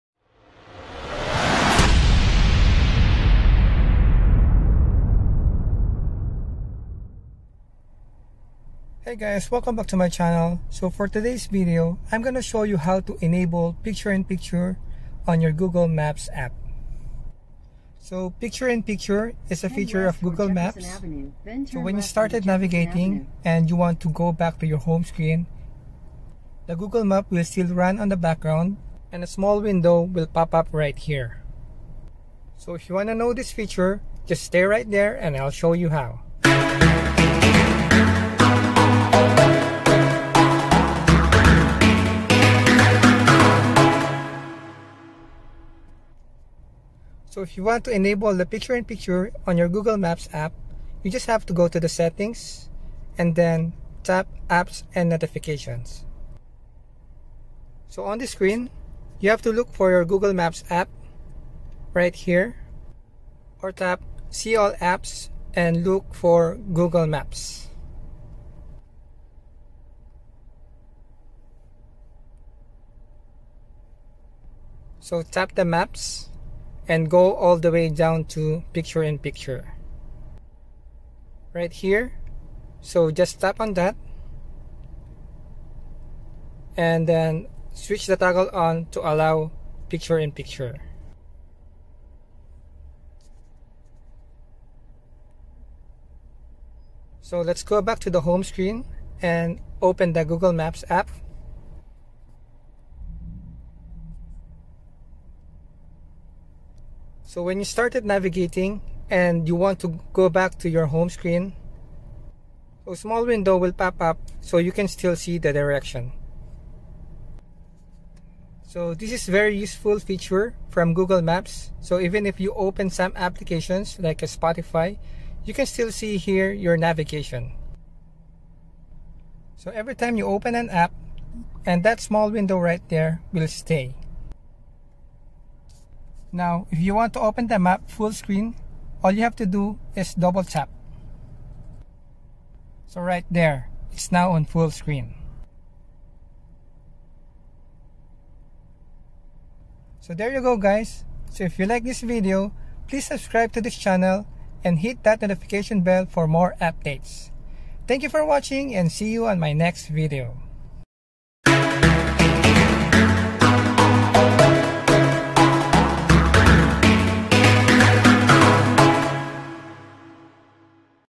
Google Maps | Enable Picture-in-Picture on Android Head Unit
Google Maps picture-in-picture feature is enabled by default on versions Android 8 and up. If it happens not turned on, watch this vid on how to enable it.